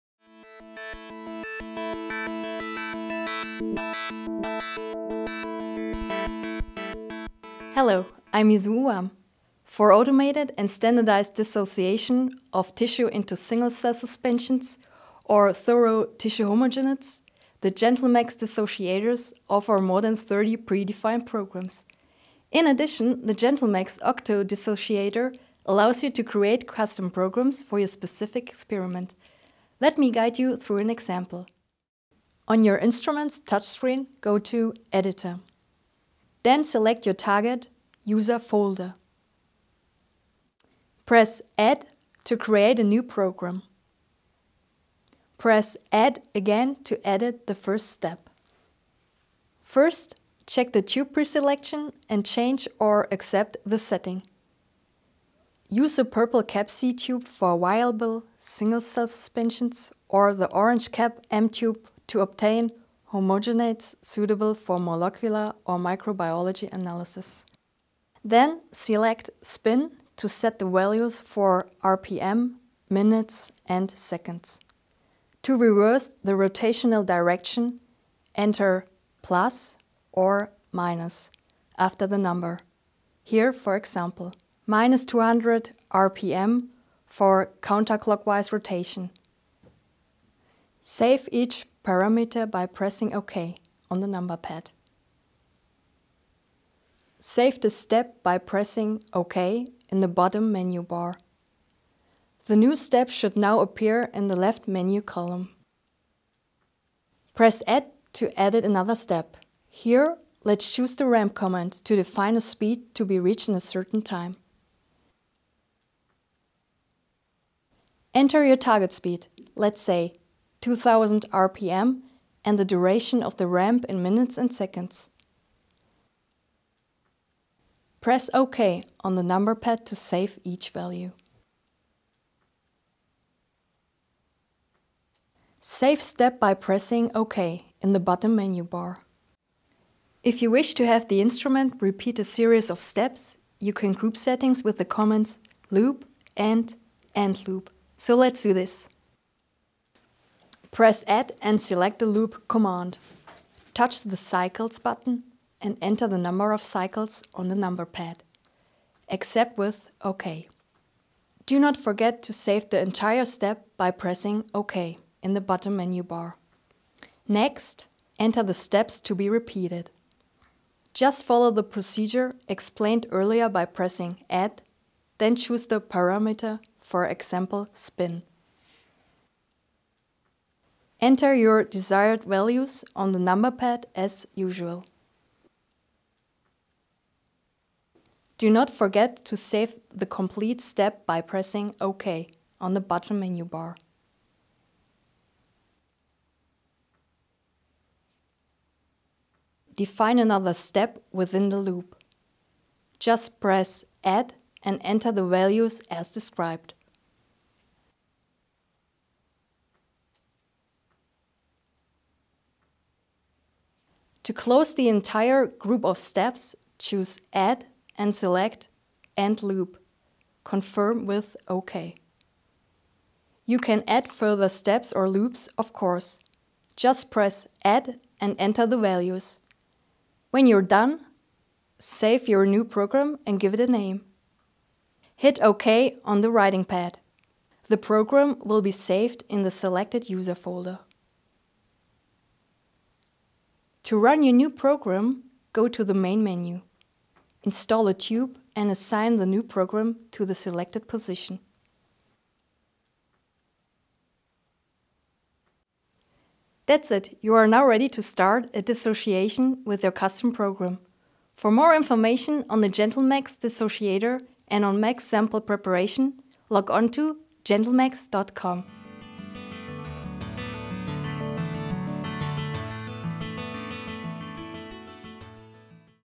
MACS Facts: Create Custom gentleMACS Octo Dissociator Programs
In addition to 30 predefined programs, the gentleMACS Octo Dissociator offers the option to create your own program for your specific experiment. This video guides you through the necessary steps.